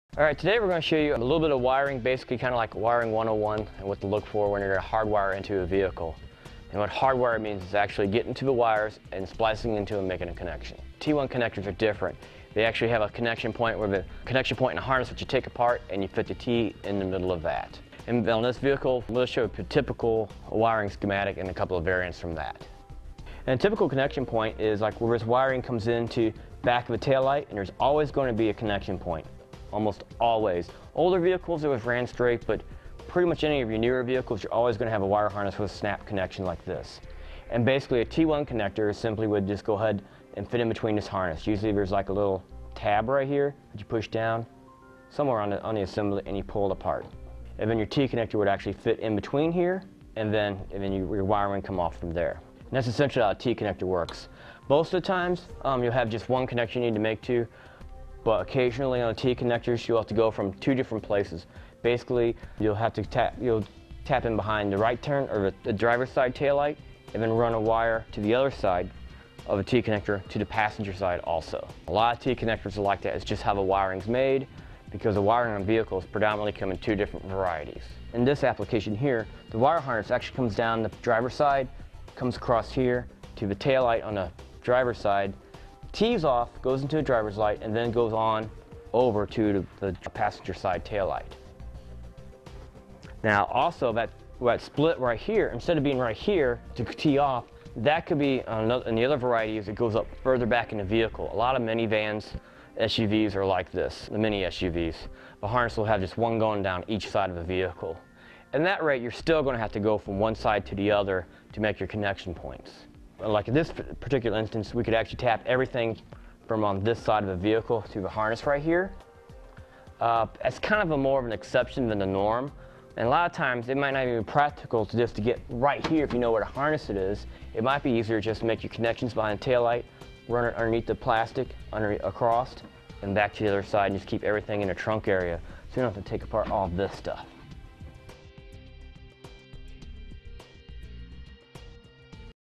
etrailer | T-One Wiring Harness Explanation
Today we are going to show you a little bit of wiring. Basically kind of like wiring 101 and what to look for when your going to hardwire into the vehicle. What hardwire means is actually getting to the wire and splicing into them to make a connection. T1 connectors are different. They actually have a connection point and harness which you take apart and fit the T in the middle of that. Now on this vehicle we will show you the typical wiring schematics and a couple variants from that. The typical connection point is like where this wiring comes into the back of the tail light and there is always going to be a connection point, almost always. Older vehicles, it would have just ran straight, but with pretty much of your newer vehicles you are always going to have a snap connection like this. Basically a T1 connector simply would just go ahead and fit in between this harness.

Usually there is a little tab right here that you push down, somewhere on the assembly, and you pull it apart. Then your T connector would actually fit in between here and then your wiring would come off from there. That is essentially how a  T connector works. Most of the time you will have just one connection that you make. But occasionally on a T connecter you will have to go from two different places. Basically you will have to tap in behind the driver side tail light and then run a wire to the other side on the T connector to the passenger side also. A lot of T connectors are like that. It is just how the wiring is made because the wiring on vehicles predominantly come in two different varieties. In this application here, the wiring harness actually comes down the driver side and comes across here to the tail light on the driver side, tees off, goes into the drivers light, and then goes on over to the passenger side tail light. Now also, that split right here, instead of it being right here to T off, that could be in the other varieties, goes up  further back into your vehicle. A lot of mini vans and SUVs are like this. The harness will have just one going down each side of the vehicle. At that rate you are still going to have to go from one side to the other to make your connection points. Like this particular instance we could tap everything from this side of the vehicle to the harness right here. It is kind of more of the exception then the norm. A lot or times it might not be practical to go right here if you know where the harness is. It might just be easier to make your connections behind the tail light, run it underneath the plastic, across and back to the other side and just keep everything in the trunk area. So you do not have to take apart all of this stuff.